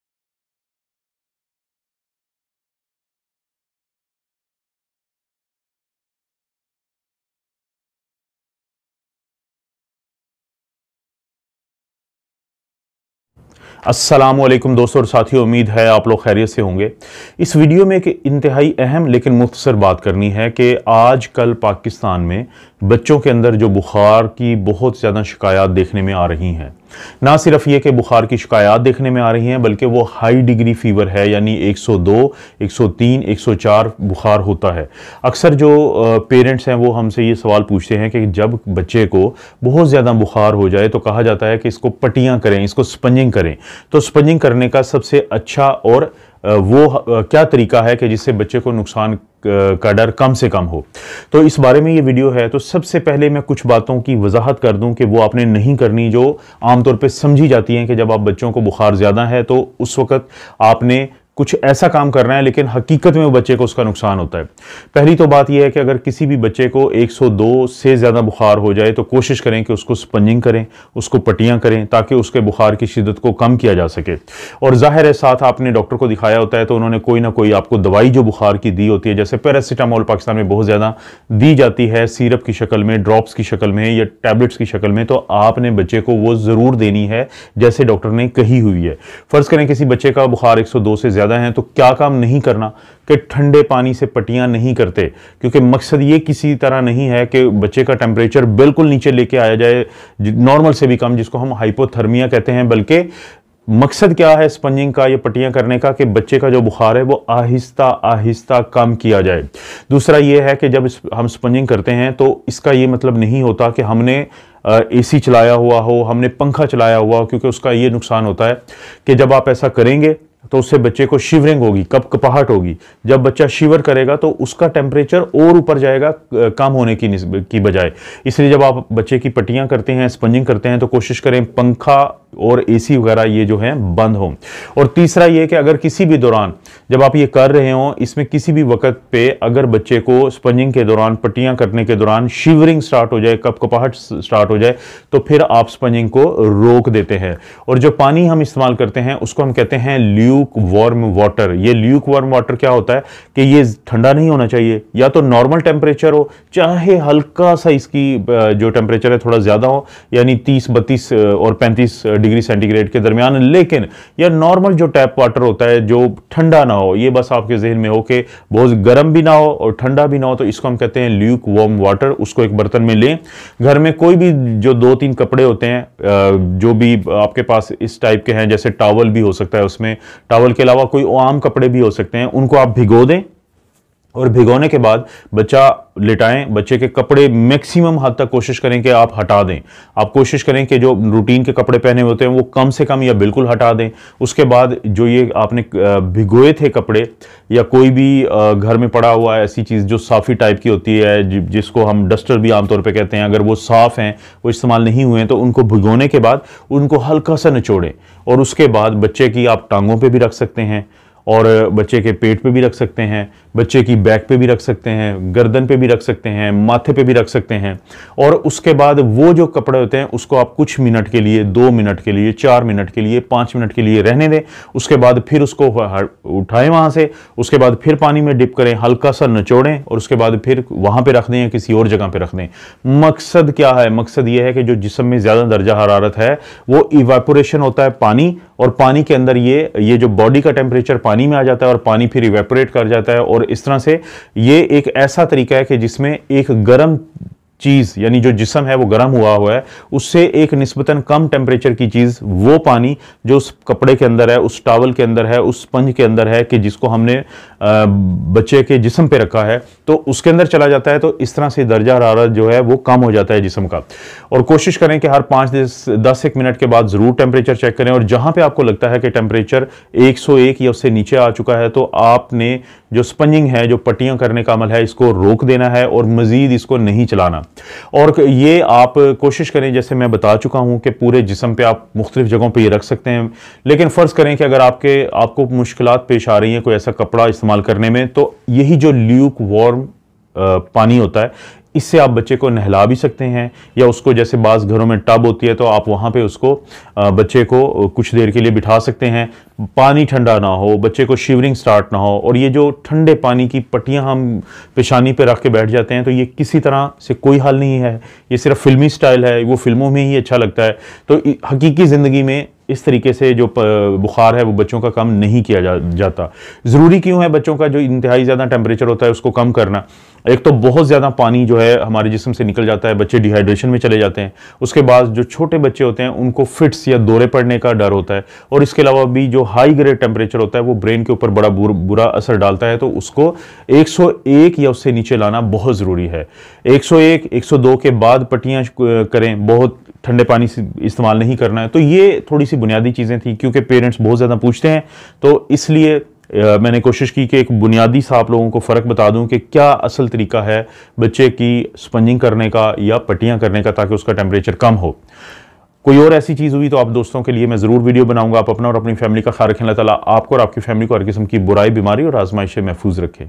How To Correctly Sponge A High Grade Fever Child?
In This Video We will Discuss How To Correctly Do Sponging In  a High Grade Fever Child in order to avoid complications